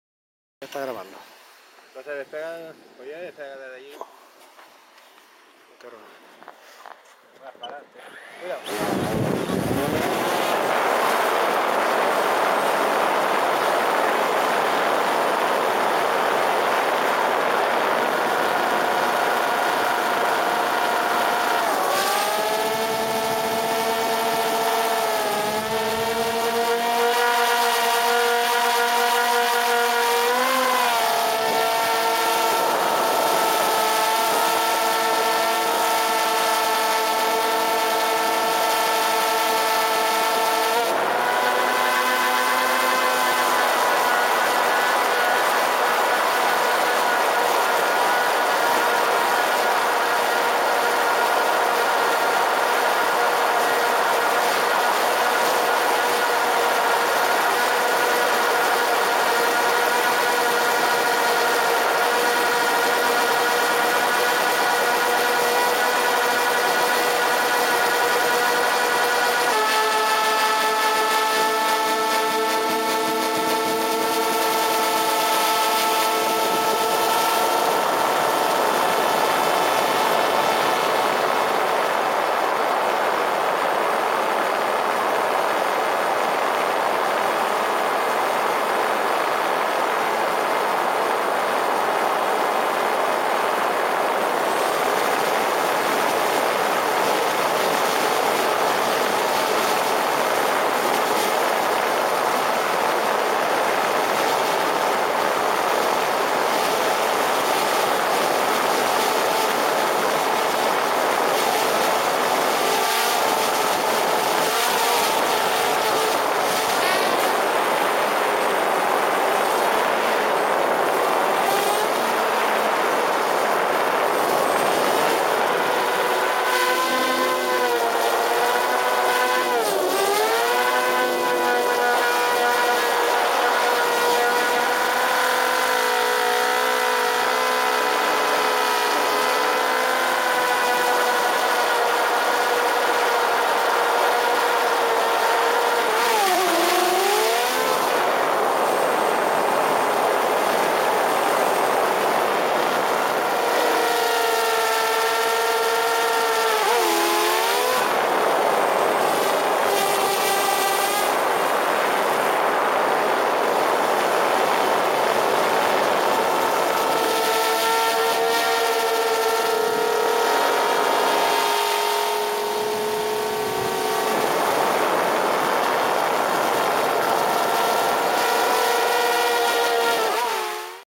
Dji spark con camara lg 360
Poderoso el spark es. Ya que puede llevar otra camara sin inmutarse jejeje